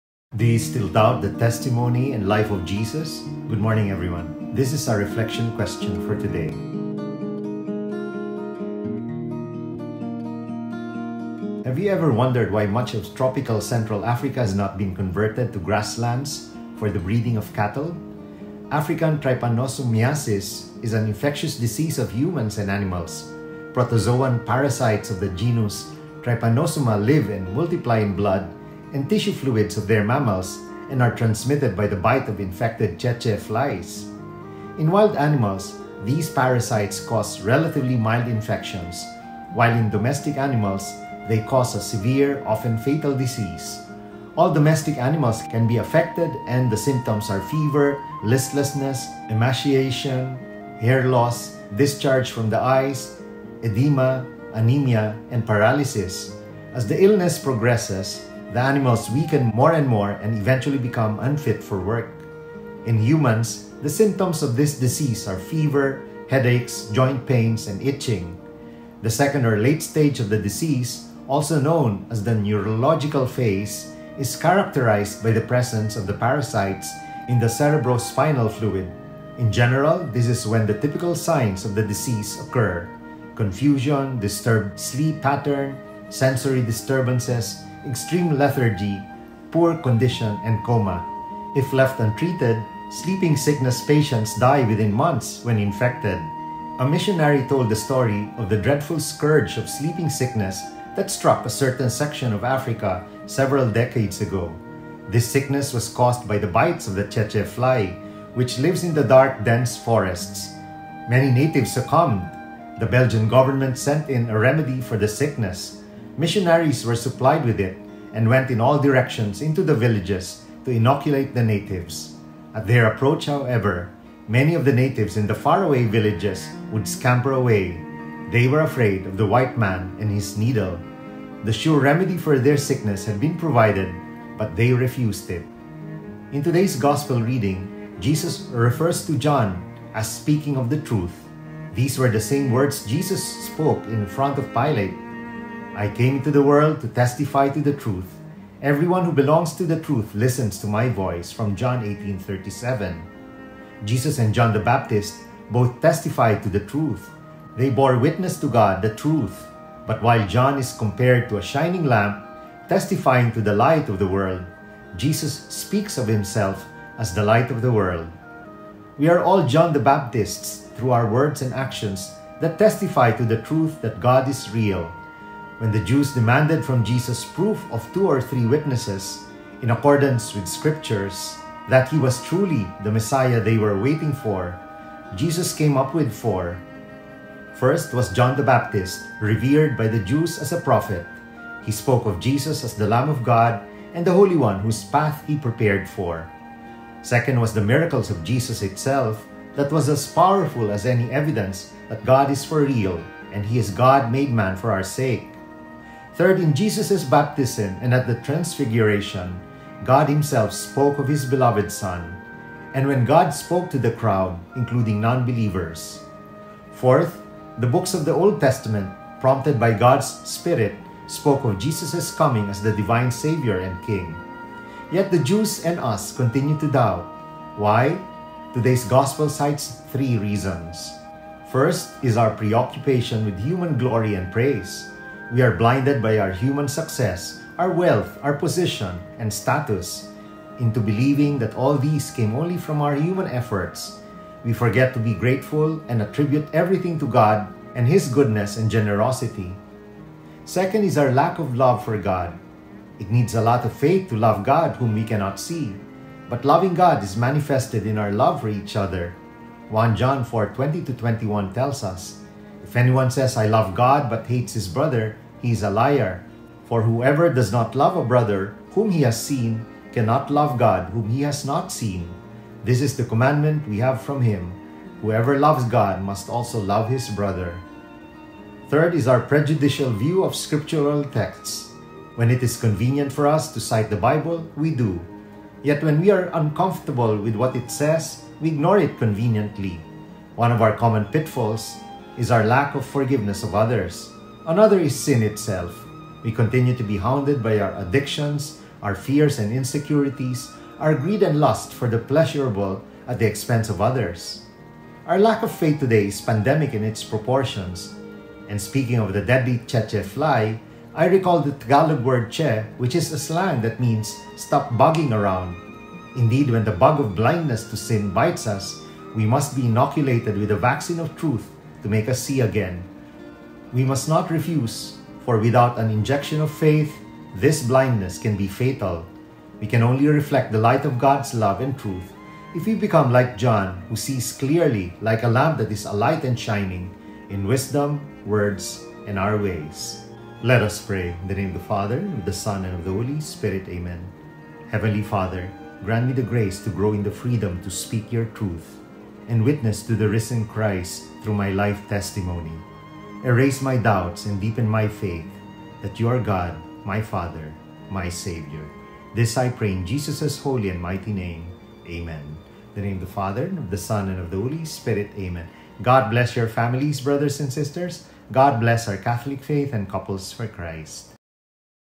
Renungan Harian CFC - 31 Maret 2022 (dengan teks Bahasa) oleh Bro. Bong Arjonillo
TOPIK: MASIHKAH ANDA MERAGUKAN KESAKSIAN DAN KEHIDUPAN YESUS?

This video belongs to Couples For Christ Global Mission Foundation Inc.
We only added Bahasa subtitles for CFC Indonesia